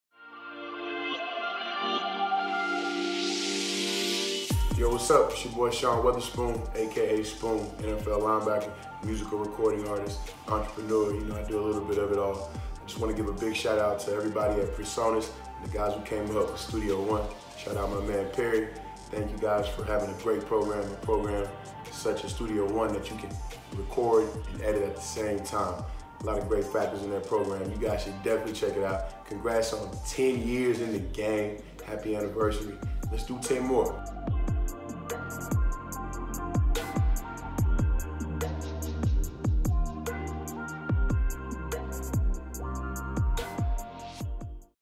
"SPOON" Gives a Shout to PreSonus!
NFL Linebacker, recording artist and Studio One enthusiast Sean Weatherspoon speaks about his experience and love for Studio One!

"Let's do ten more!" It's on!